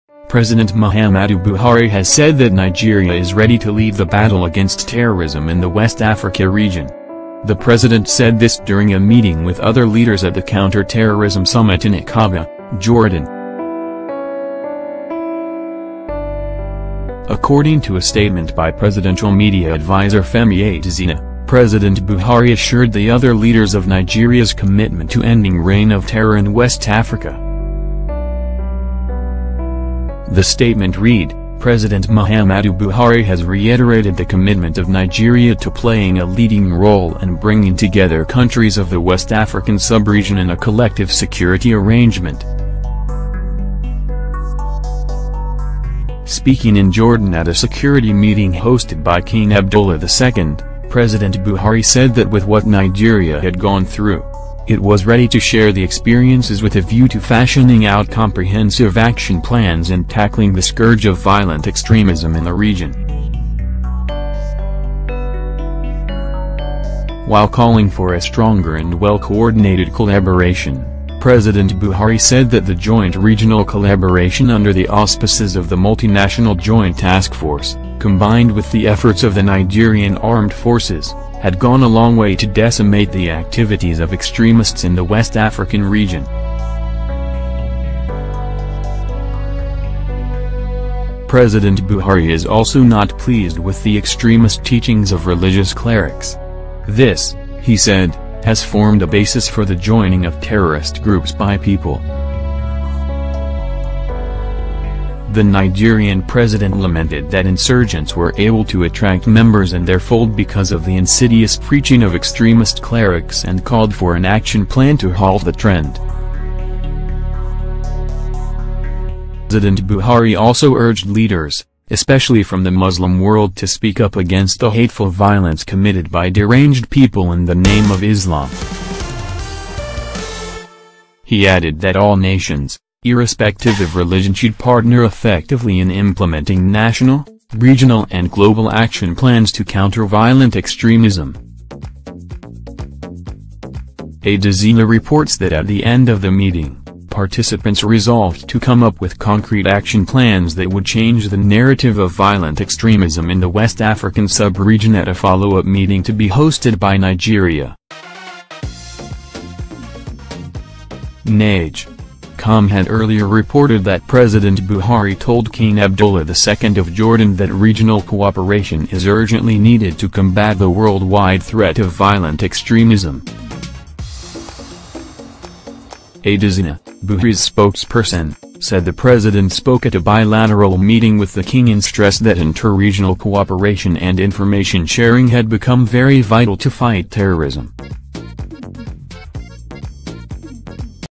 Nigeria will lead the way in fight against terrorism in West Africa - Buhari
- President wants measures to be taking against extremist preaching by religious clerics in the West Africa sub region

- Presidential spokesman Adesina said Buhari also promised Nigeria's commitment to leading the anti-terrorism campaign the region

- President Buhari said all these at a meeting with other leaders at the counter-terrorism summit in Aqaba, Jordan